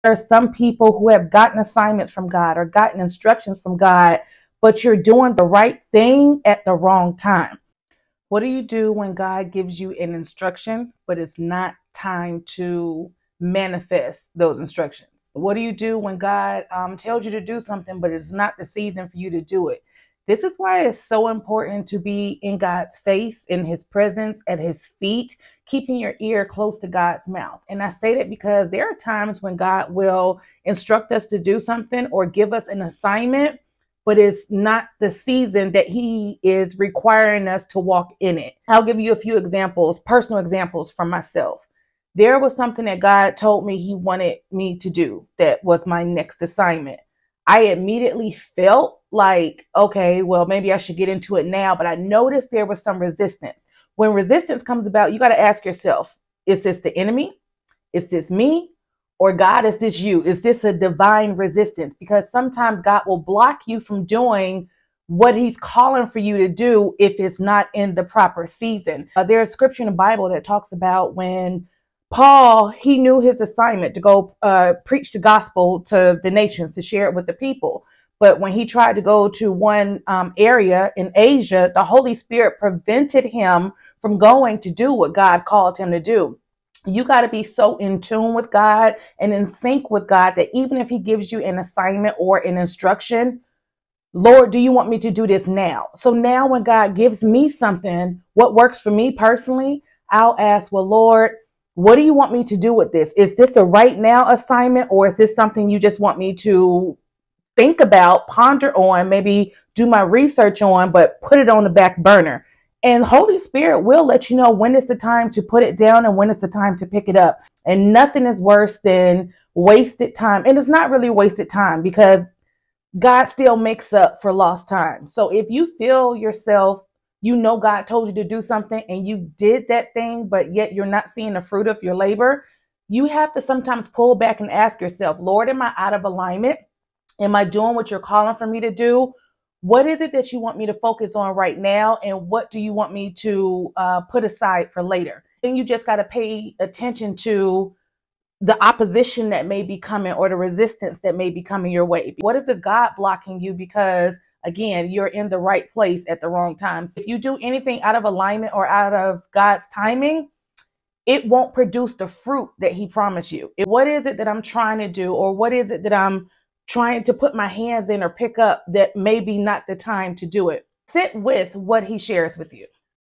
Obedience in the WRONG season 🥴
Yes God gives us assignments and instructions to produce fruit in those assignments, but we must learn our seasons and understand God’s timing!